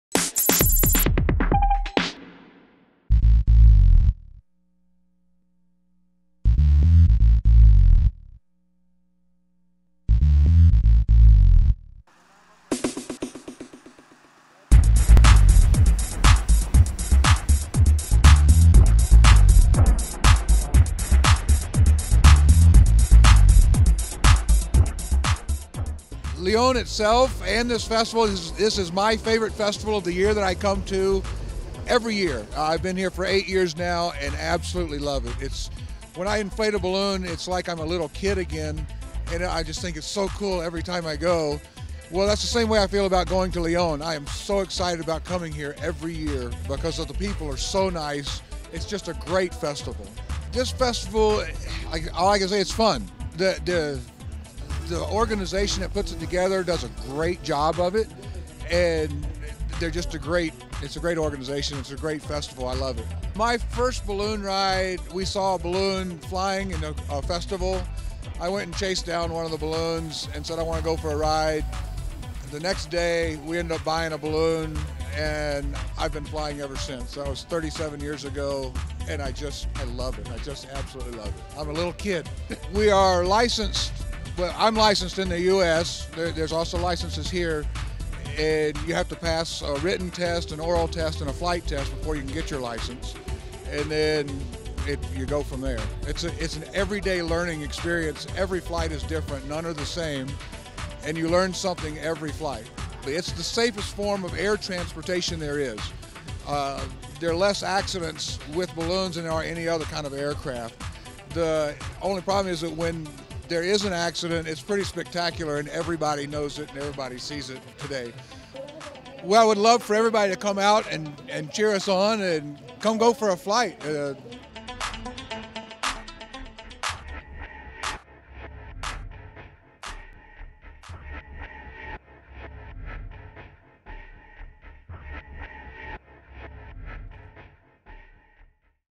Volar en globo, una experiencia extraordinaria y segura: Stuart Enloe
Con la menor cantidad de accidentes en el aire, los globos aerostáticos son el transporte aéreo más seguro que existe